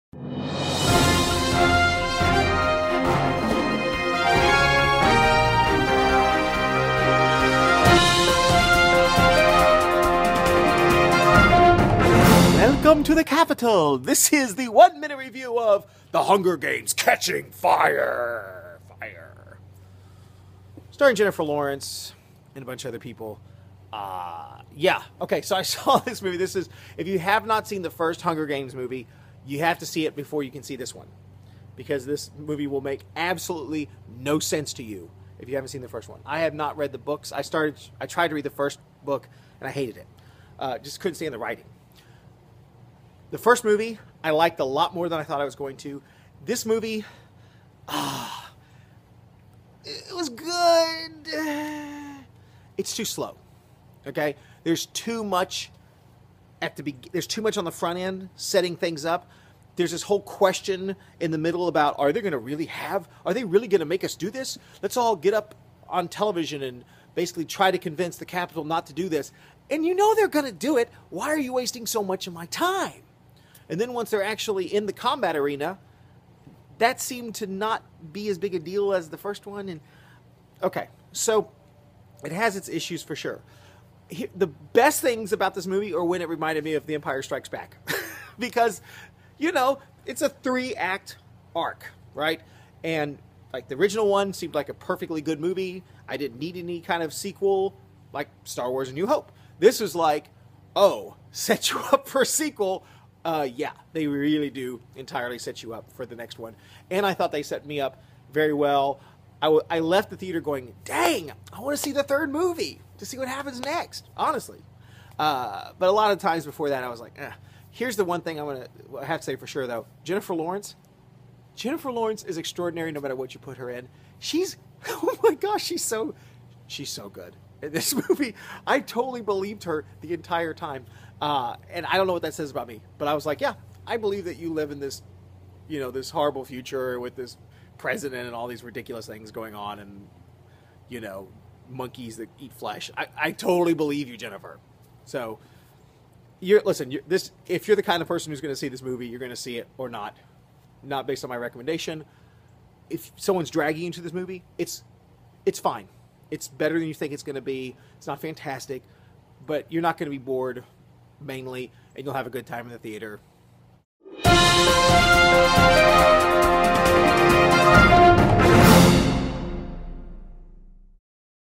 One Minute Review: Catching Fire
Fr. Thomas McKenzie returns to Panem for the Hunger Games: Catching Fire. Should you go see it, or will you just get burned? www OneMinuteReview com